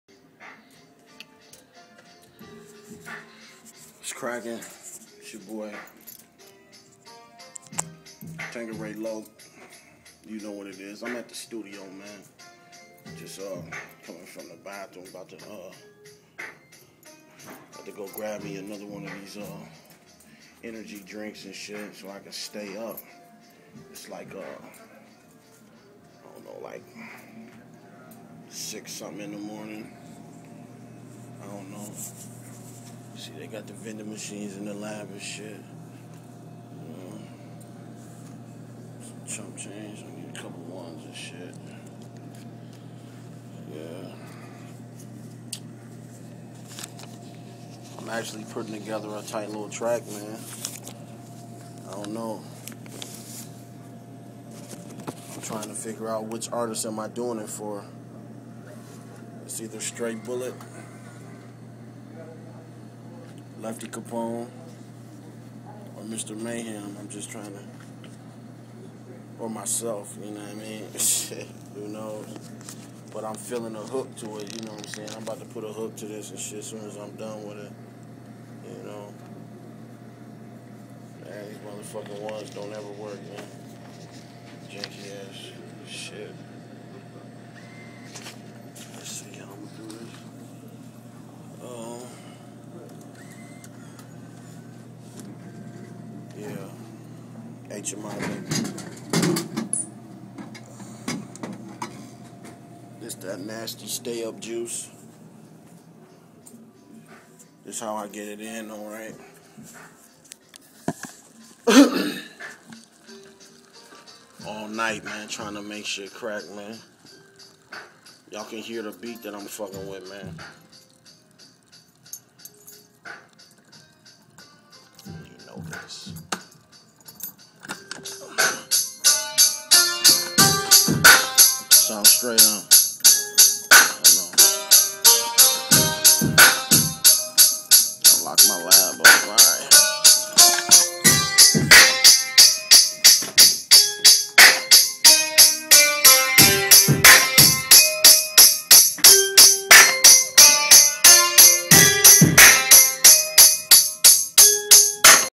At the lab in the am
Tanqueray Loccster at his Heated Muzic Inc studio in the am fuccin around with beats